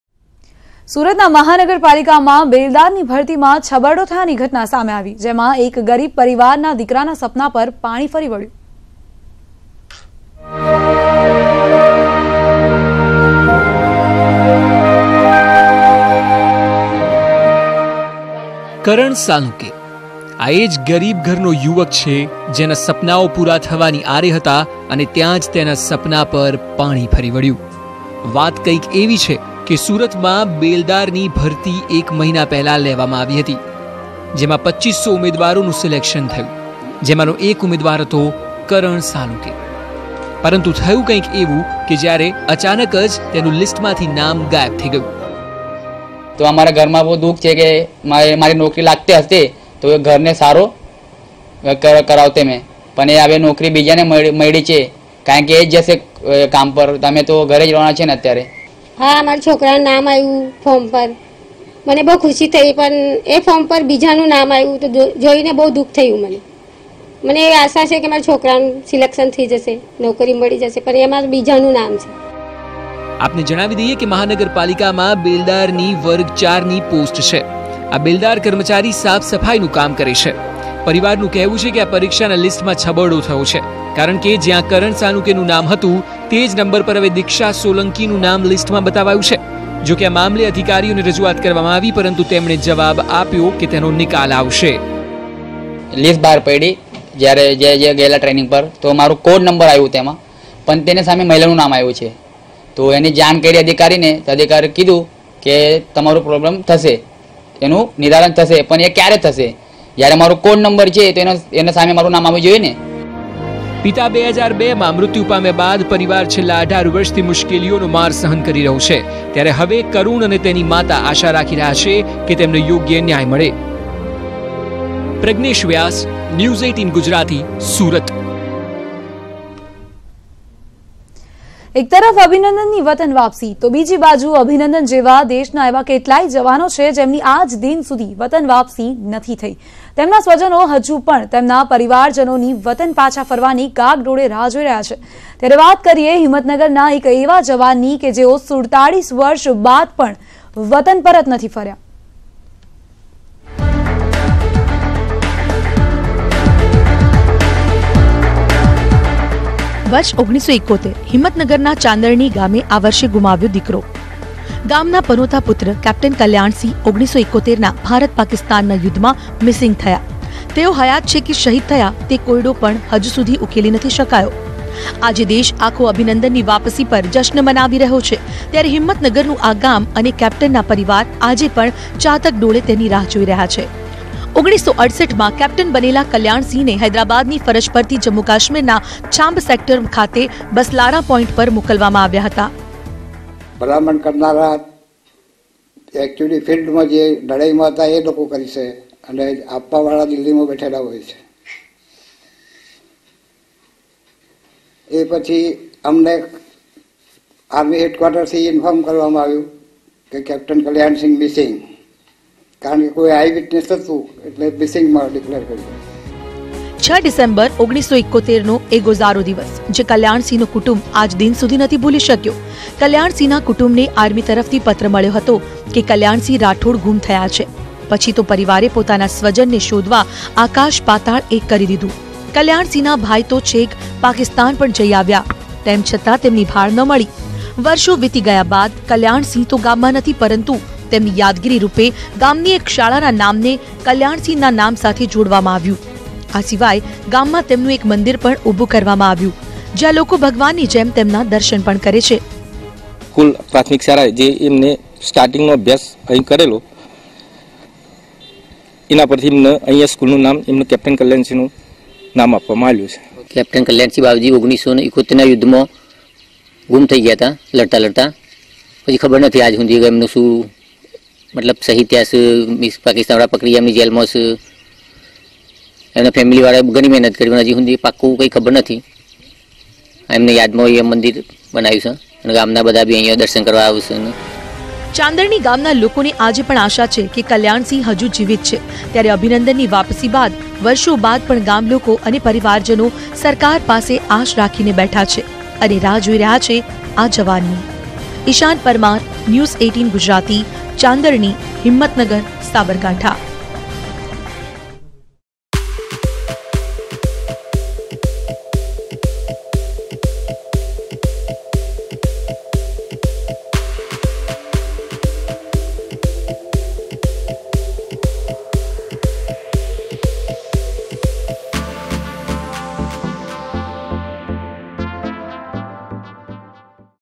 સુરત: ગરીબ પરિવારના દીકરાનું નામ અચાનક બેલદારની ભરતીના લિસ્ટમાંથી થયું ગાયબ
સુરત: ગરીબ પરિવારના દીકરાનું નામ  અચાનક બેલદારની ભરતીના  લિસ્ટમાંથી થયું ગાયબ, વધુ જાણકારી માટે જુઓ પૂરો વિડિઓ।